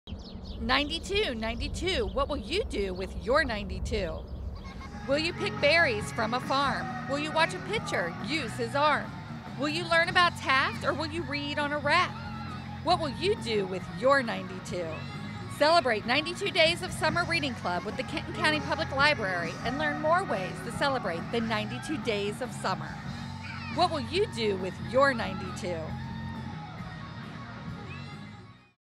92 Days Summer Reading Club Kenton County Public Library 2015
92 days Summer Reading Club Kenton County Public Library 2015 giveaway!  Join the Summer Reading Club at all 3 branches of the Kenton County Public Library for events, prizes and loads of summer fun!!!  Adults, teens and children welcome!!!

Produced by the Kenton County Public Library PR/Dev't Department.